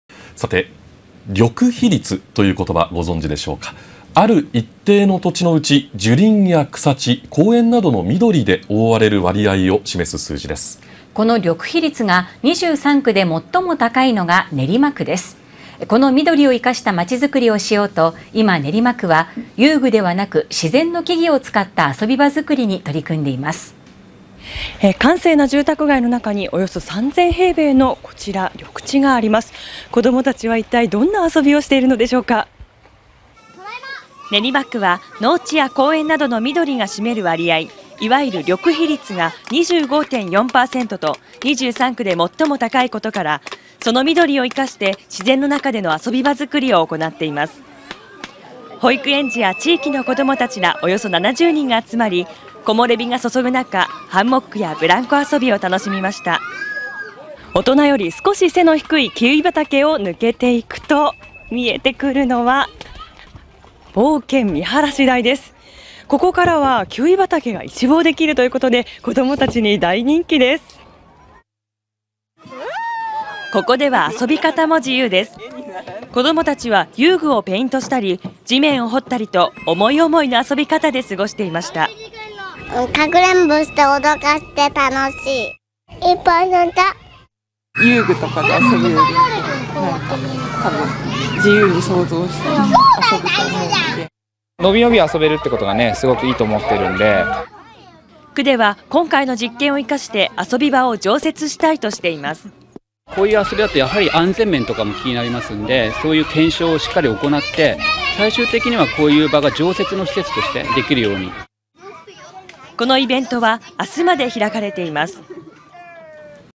緑被率No.1の練馬区 自然の遊び場づくり実証実験スタート
「緑被率」という言葉をご存知でしょうか。ある一定の土地のうち、樹林や草地、公園などの緑で覆われる割合を示す数字です。この「緑被率」が23区で最も高い練馬区は緑を生かした街づくりをしようと、今、遊具ではなく自然の木々を使った遊び場づくりに取り組んでいます。
　練馬区は、農地や公園などの緑が占める割合＝「緑被率」が25.4％と23区で最も高いことから、その緑を生かして自然の中での遊び場づくりを行っています。閑静な住宅街の中に広がるおよそ3000平方メートルの緑地では、保育園児や地域の子どもたちおよそ70人が集まり、木漏れ日が注ぐ中、ハンモックやブランコ遊びを楽しみました。
　大人の背丈よりも
■■↓全文を見る■■